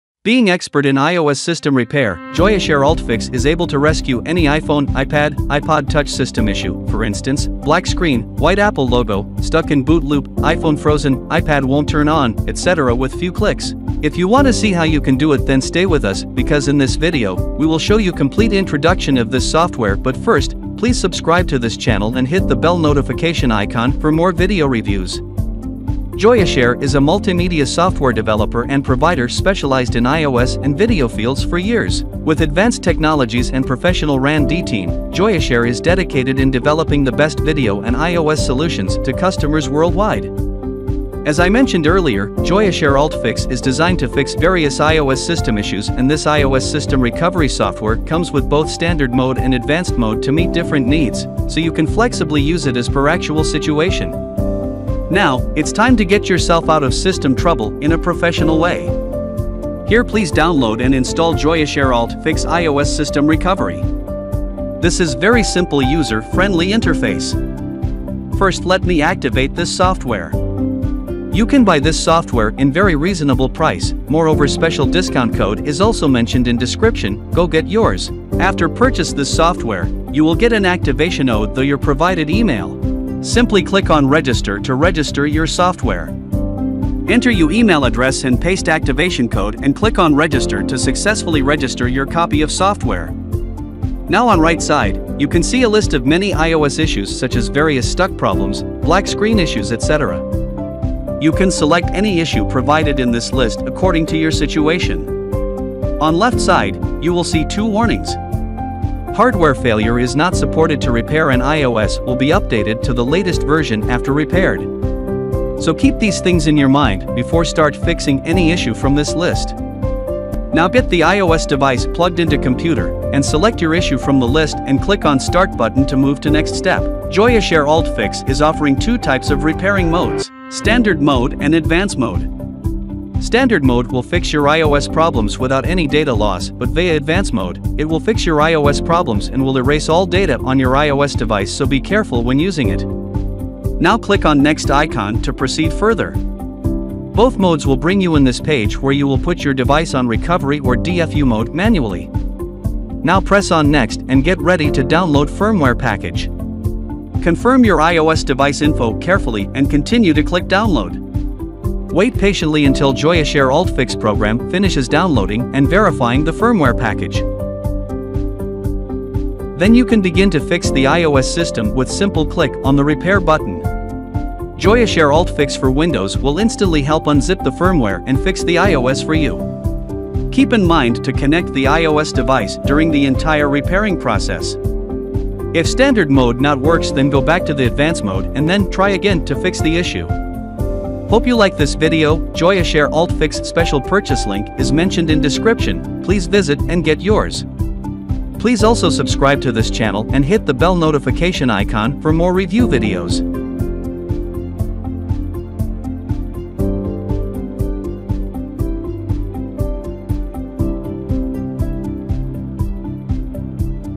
Fix iPhone Stuck on Apple Logo without Data Loss | Joyoshare UltFix iOS System Recovery
Joyoshare UltFix iOS System Recovery to fix iPhone/iPad/iTunes errors with No data loss.